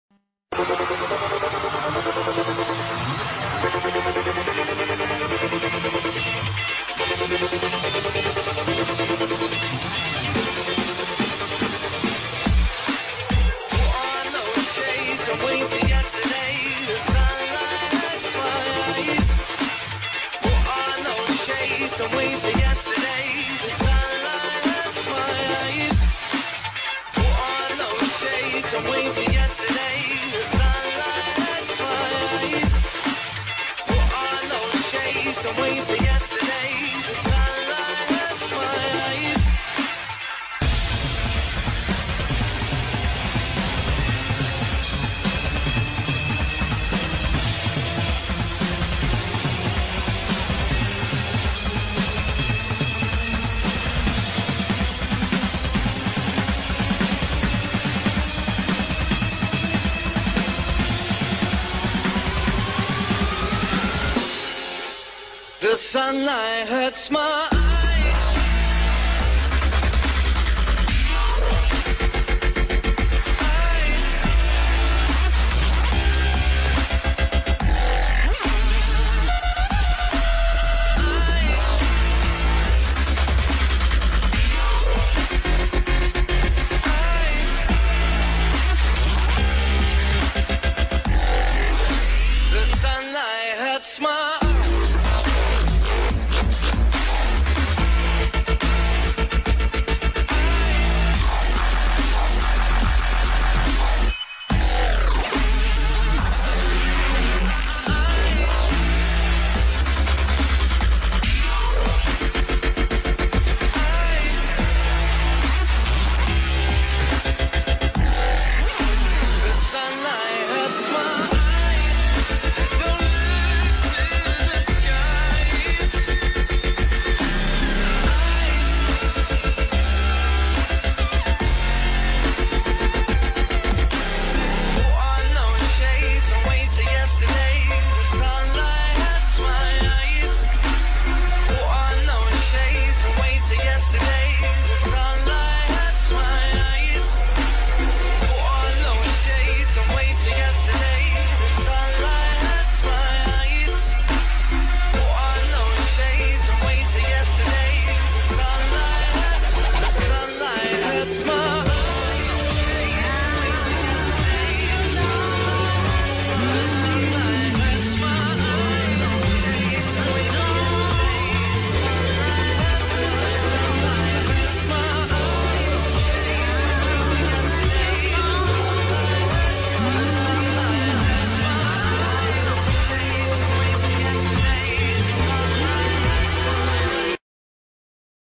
carpark 3inch 3am
Late nite mission coz i missed the weekend thanks to the runny shits hahaha.